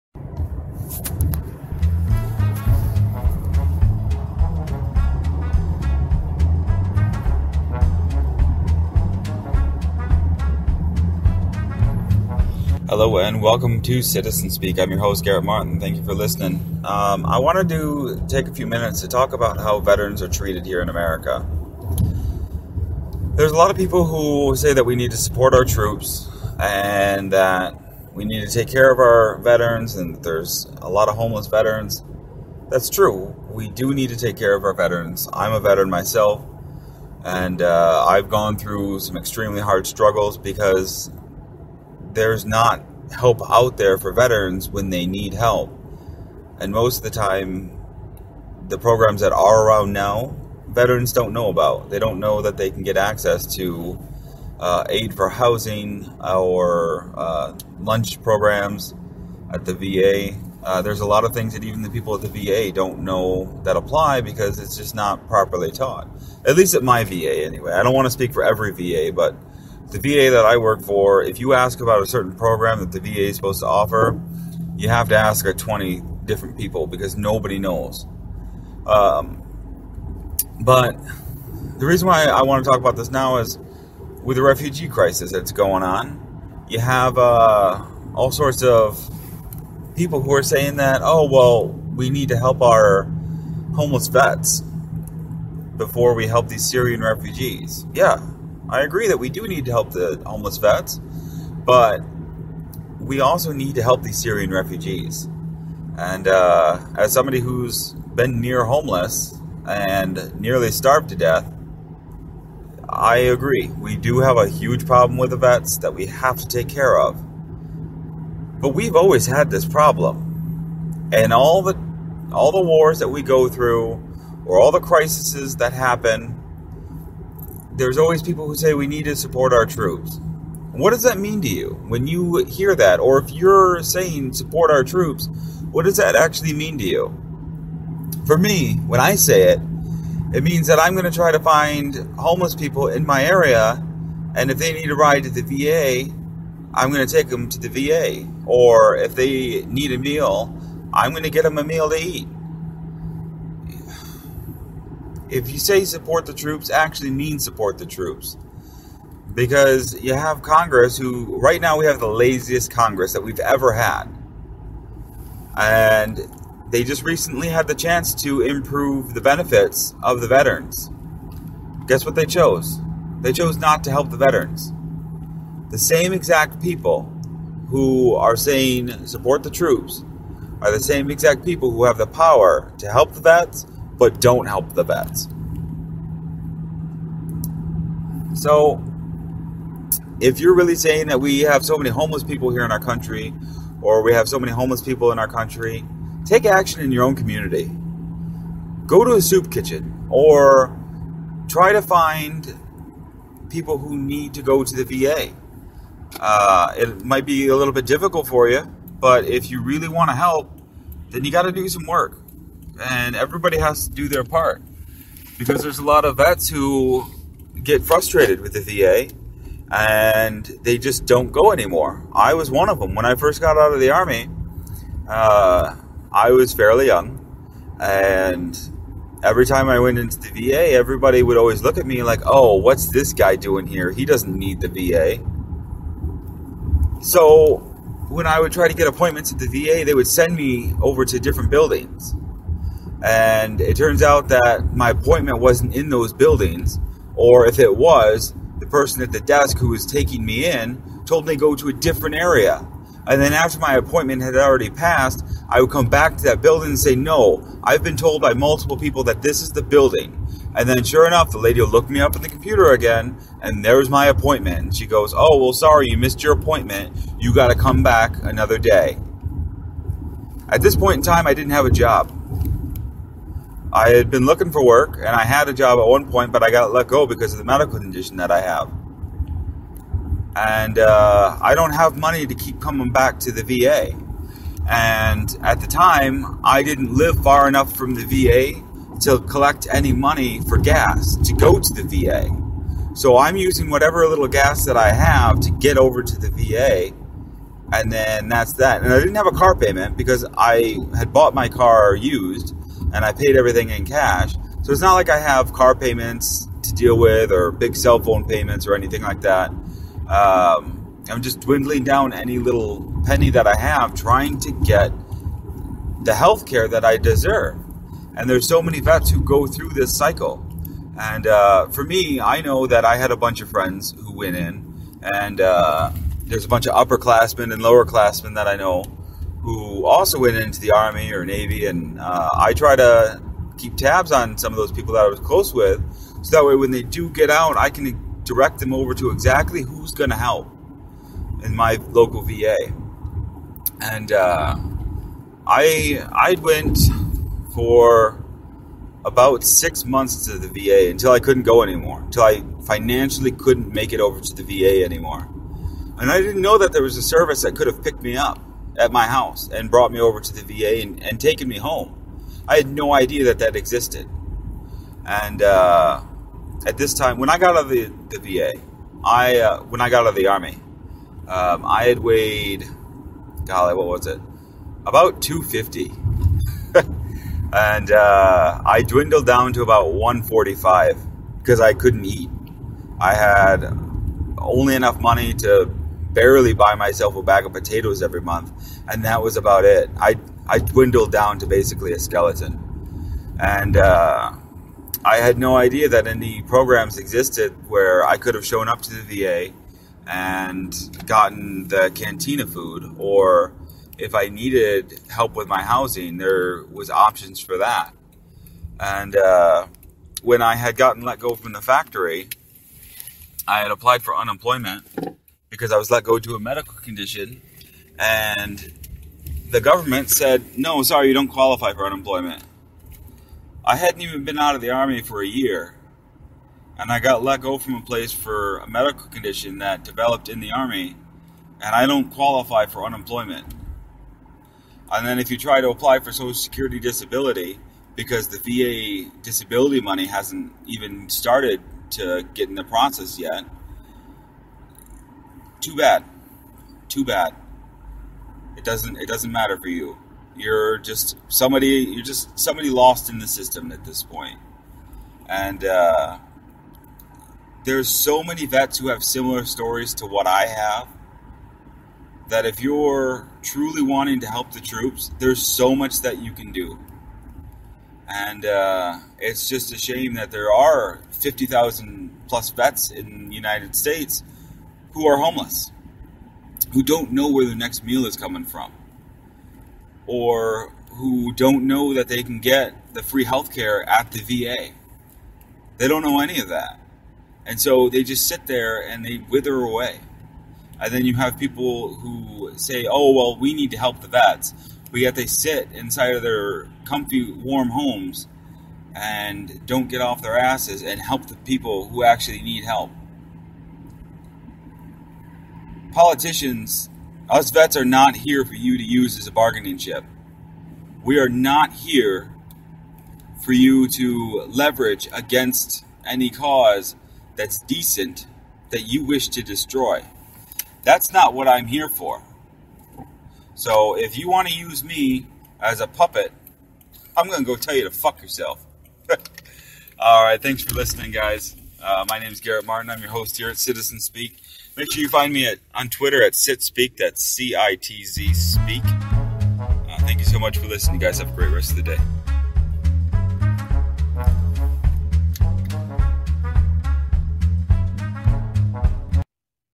Support Our Troops
Our politicians need to quit being Un-American and start supporting our troops.